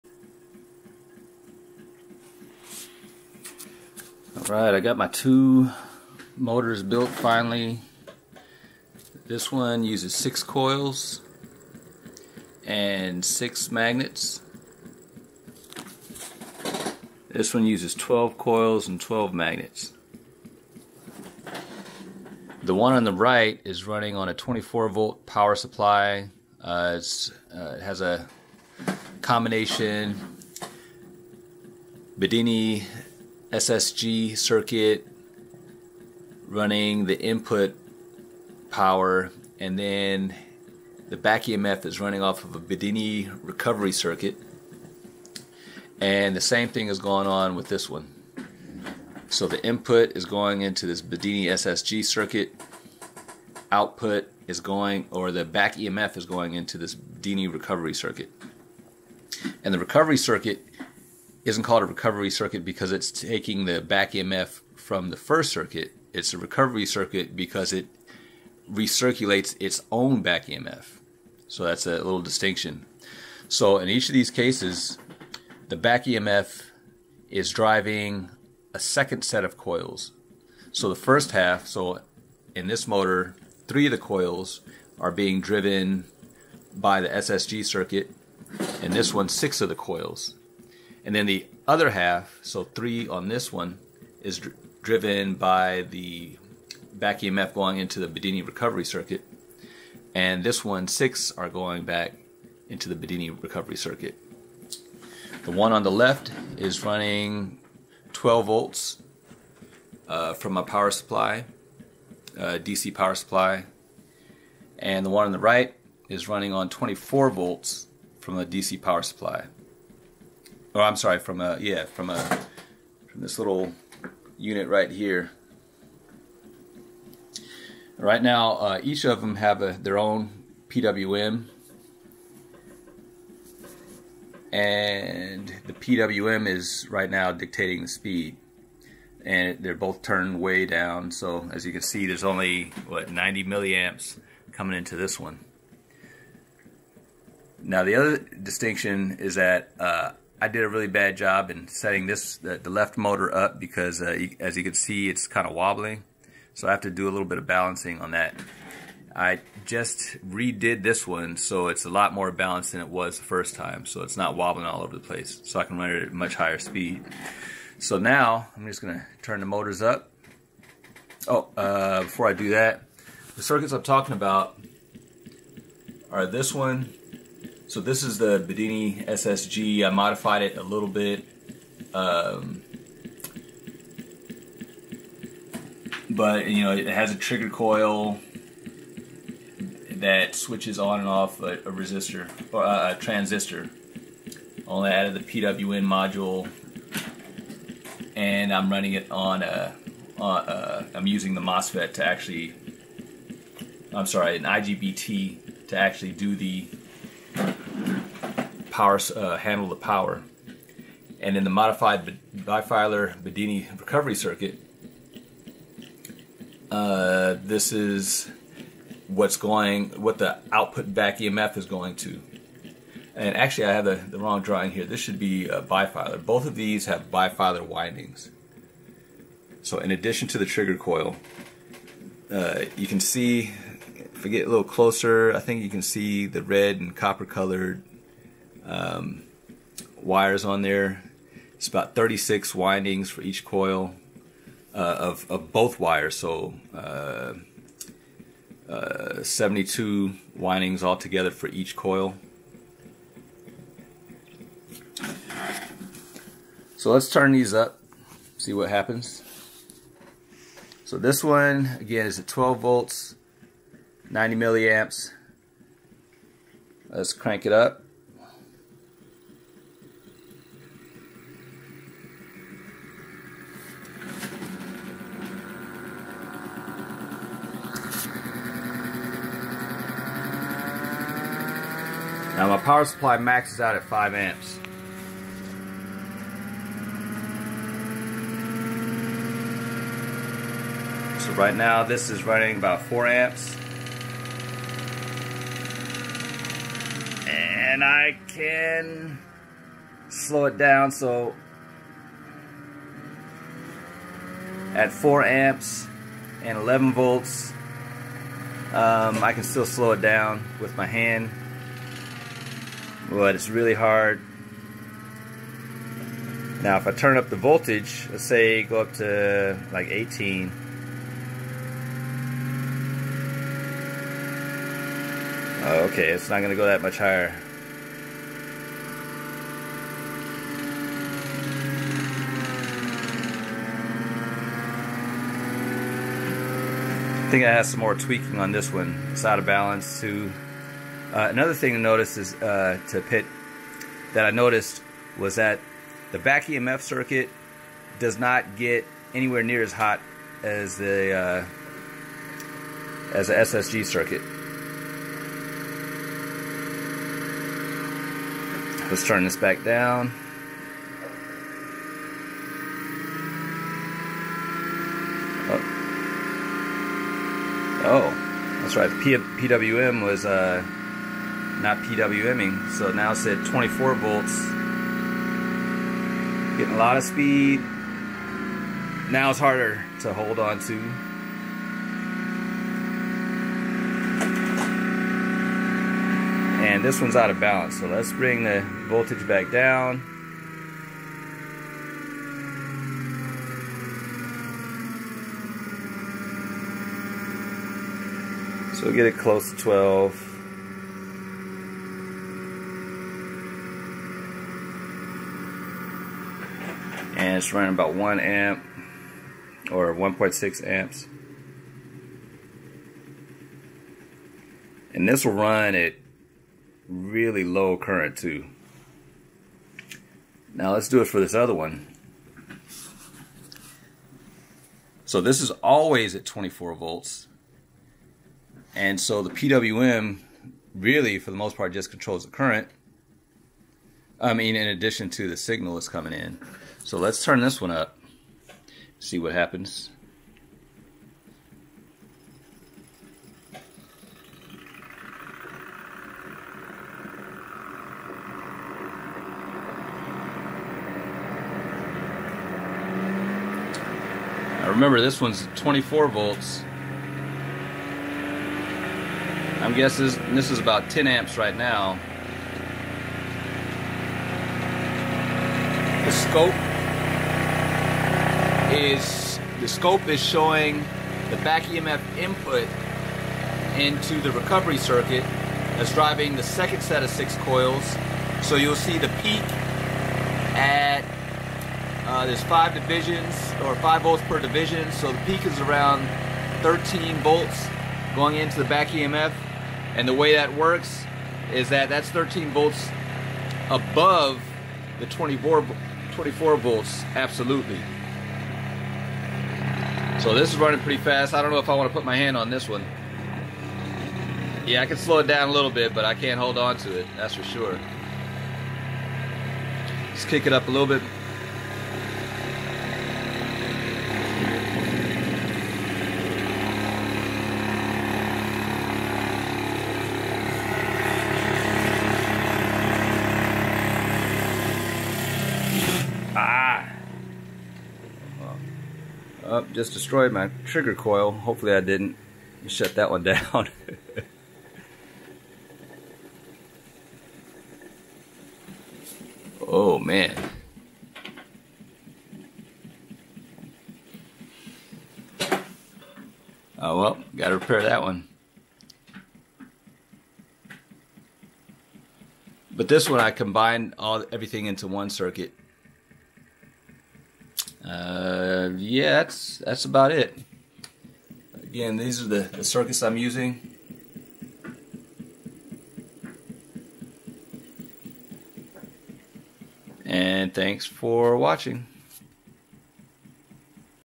Catastrophic Trigger Coil Failure - 2 DIY Hand Built Axial Flux BLDC Motors
A 6 pole BLDC air core motor using a modified version of the Bedini SSG circuit for one bank of coils (3), and a Bedini Recovery circuit to run the other bank of coils with the back EMF of the first bank serving as its power input. A 12 pole BLDC air core motor with one bank of coils (6) run a modified Bedini SSG circuit, and the others run on a Bedini Recovery circuit where the back EMF from bank 1 services as the input.